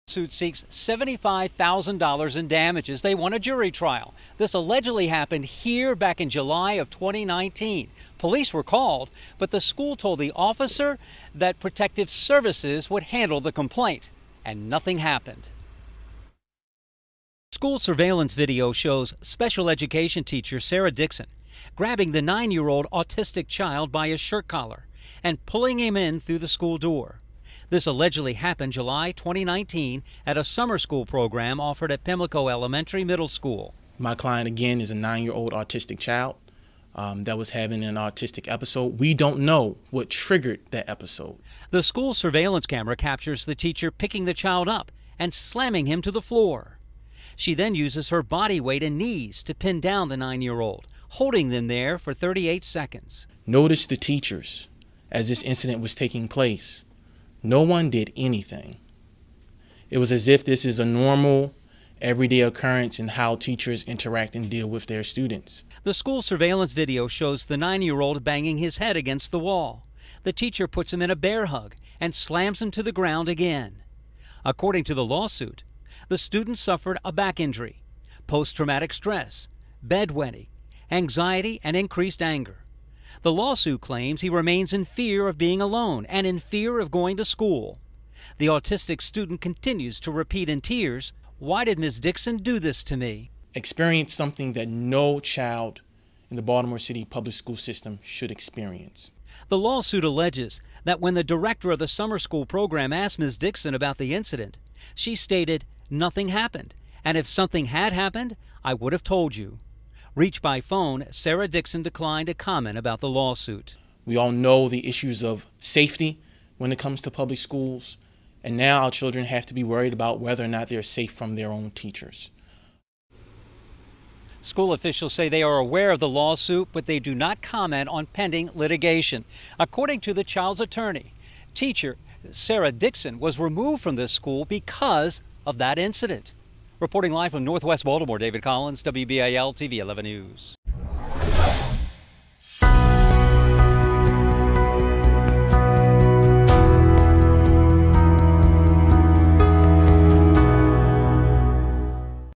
Lawsuit alleges abuse of boy with autism by former teacher
A parent of a 9-year-old student with autism is suing the Baltimore City Board of School Commissioners and a former special education teacher.